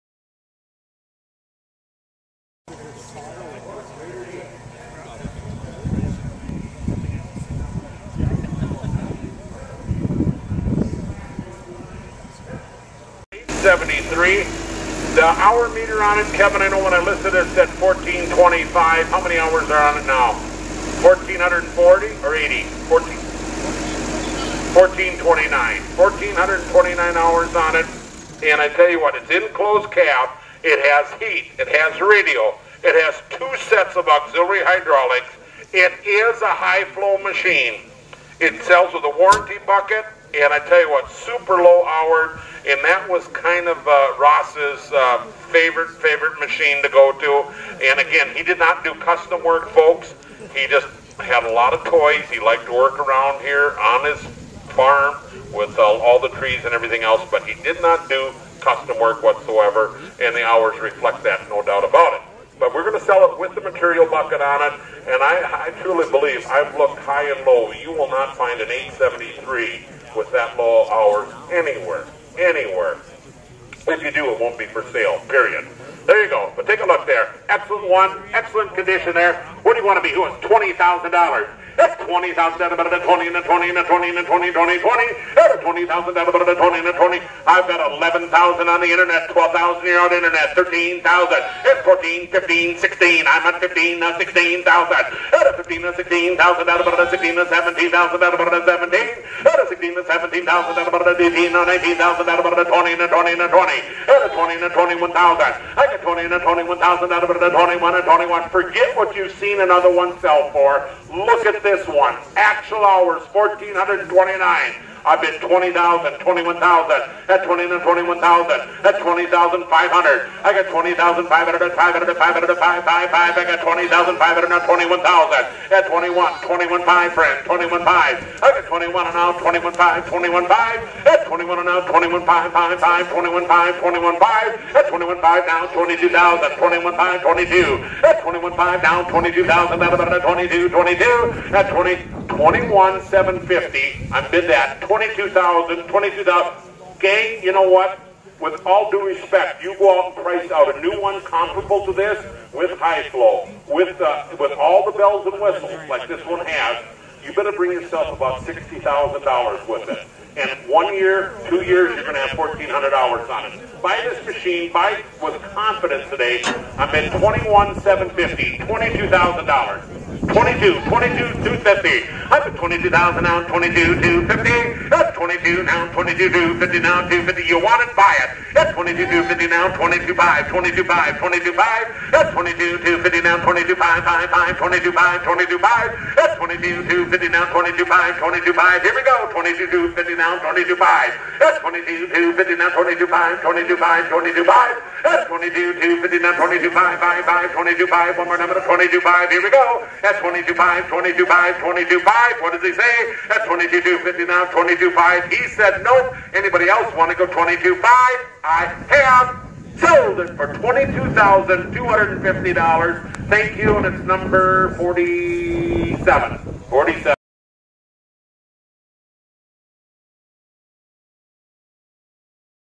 2001 Bobcat 873 Skid Steer with 1,425 Hours Sold on Minnesota Farm Auction
Machinery Pete shoots video of a 2001 Bobcat 873 skid steer with 1,425 hours selling on a November 16, 2013 farm estate auction in southeast Minnesota